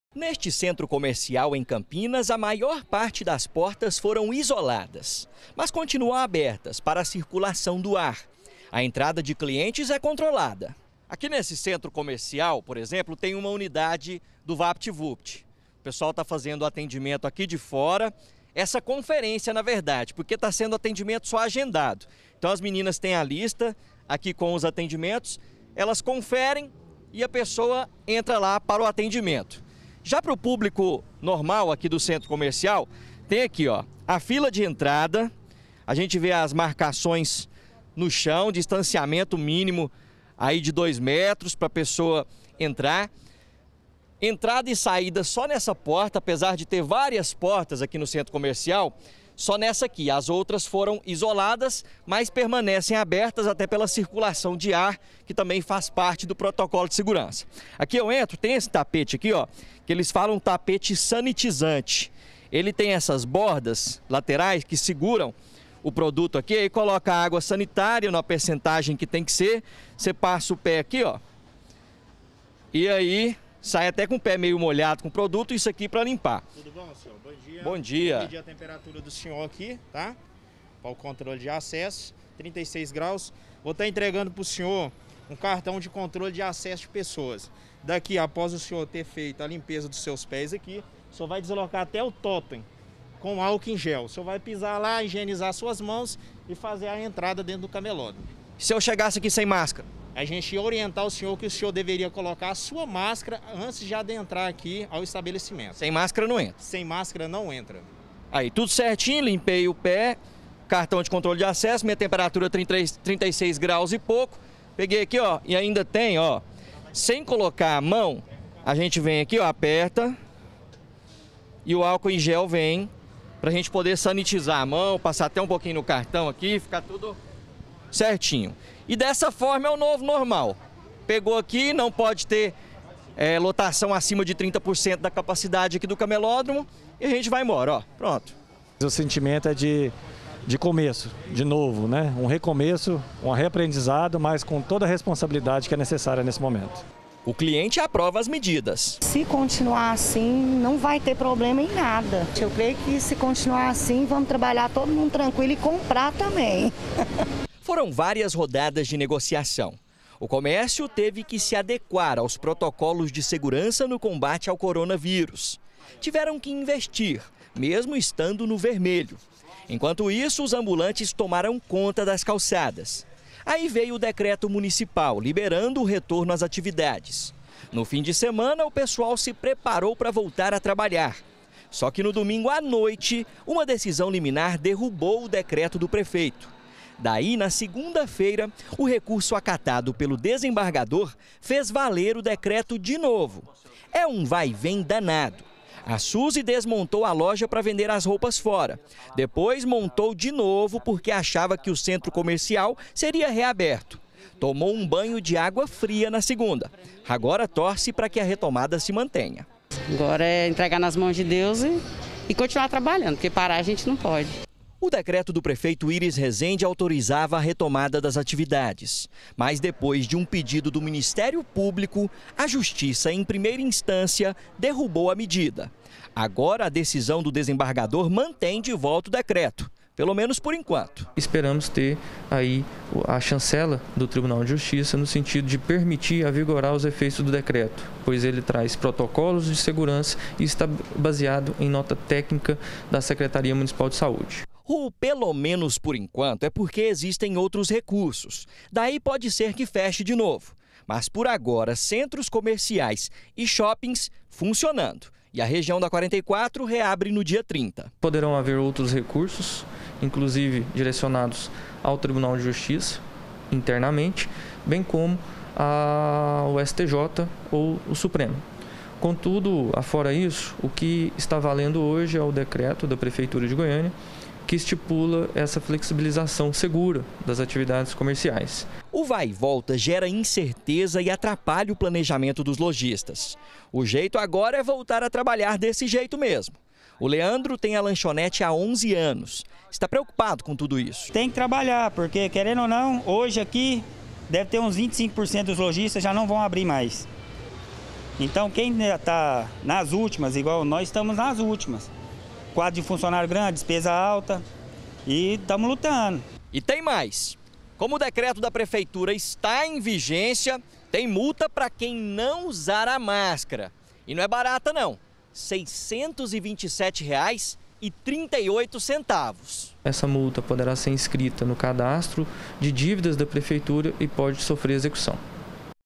NOVA DECISÃO DA JUSTIÇA PERMITE A REABERTURA DO COMÉRCIO
Muitos comércios voltaram a funcionar em Goiânia. Comerciantes e clientes começam a se adaptar as novas regras de convivência para combater a pandemia.